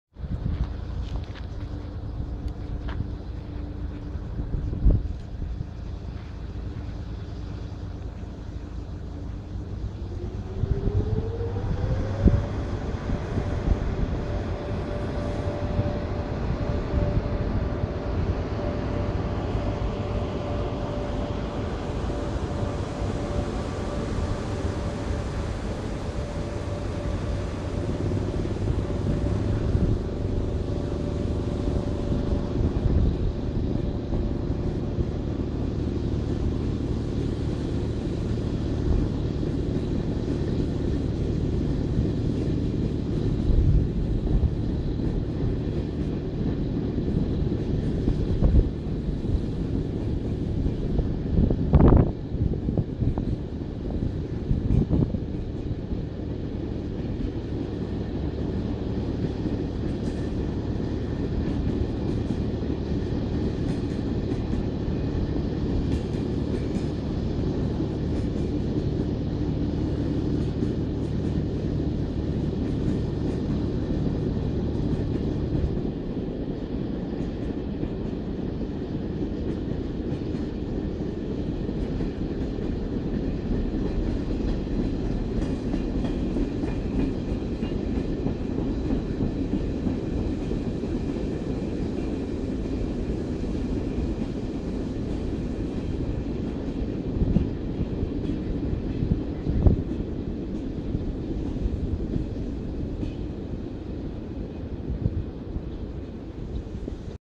T 3722 lähtee Raumalta
Syitä tilata RVS:n kanava Youtubessa:
- Monipuolisesti kaikkea rautateistä! Tyhjä rapujuna T 3722 lähtee Raumalta Tampereelle kello 19:47